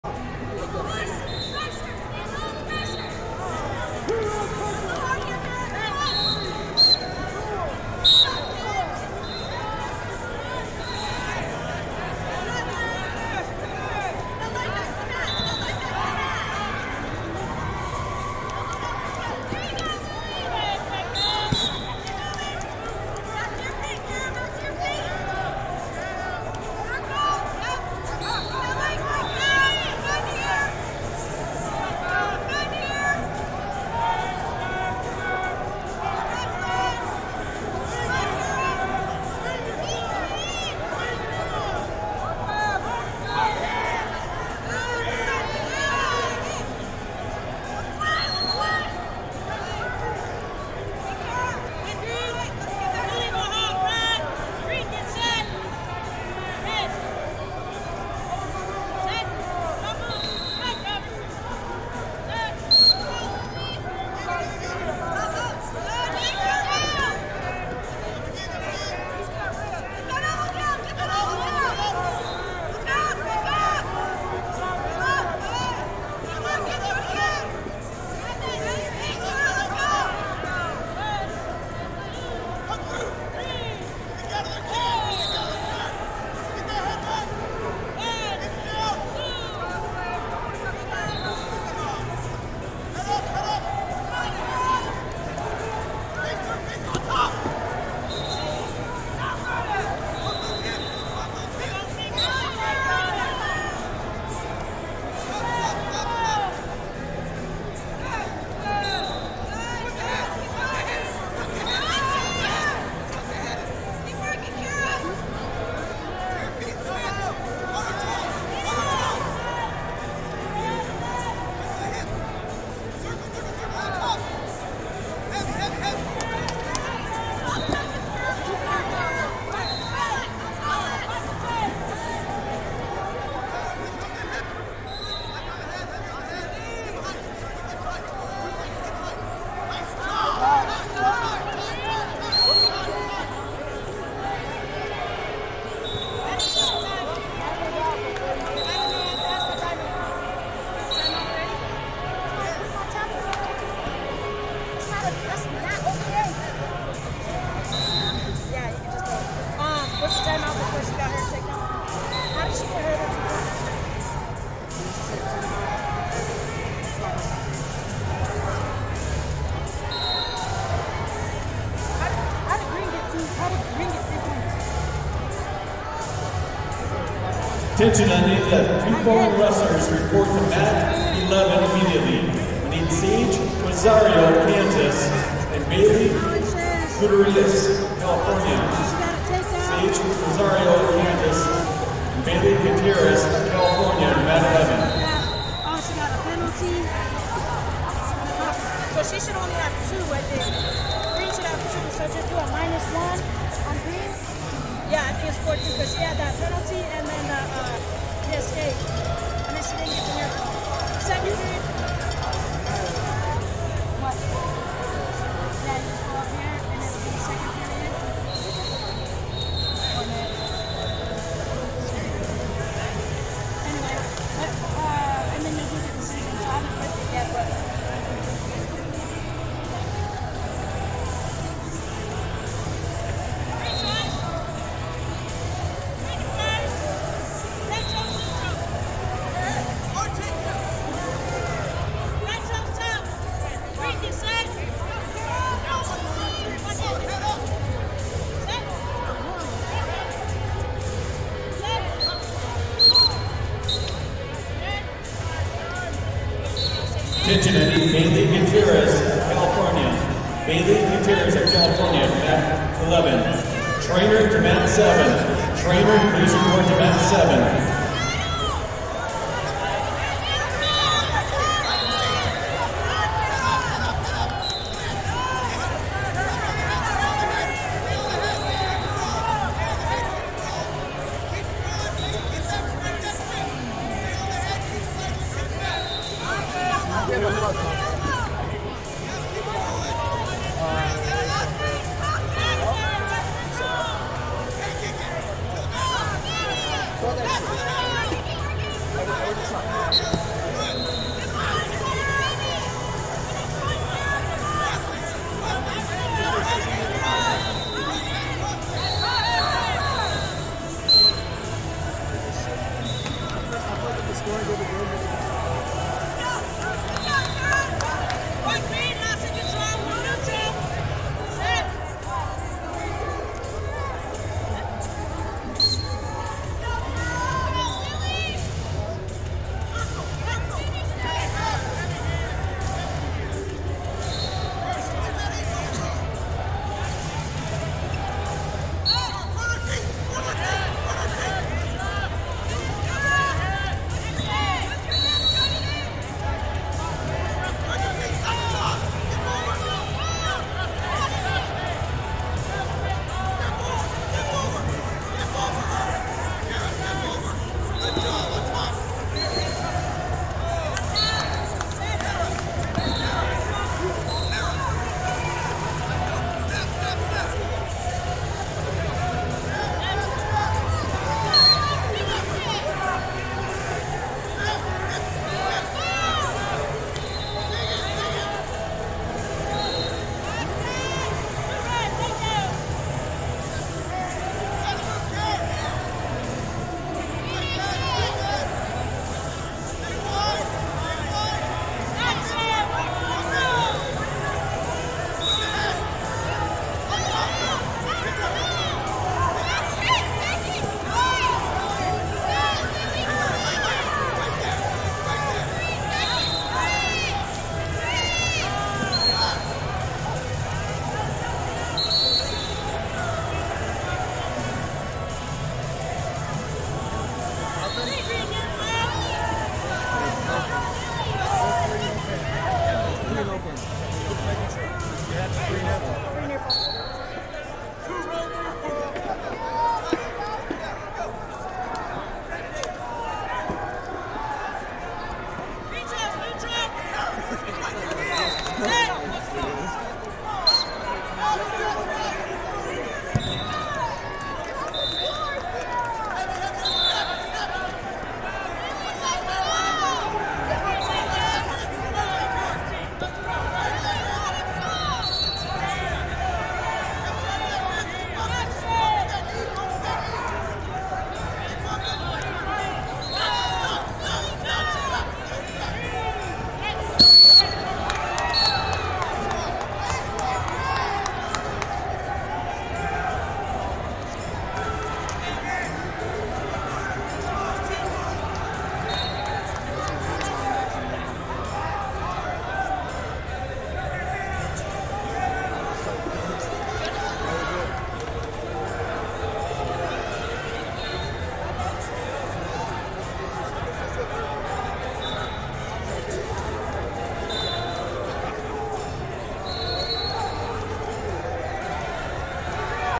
138 Lbs Cons. Round 1 - Kara Noyce, Utah Vs Lily Fitzpatrick, Montana 4c17
FloSports/2022 Girls Folkstyle National Championships & Junior Women's Duals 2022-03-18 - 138 Lbs Cons. Round 1 - Kara Noyce, Utah Vs Lily Fitzpatrick, Montana 4c17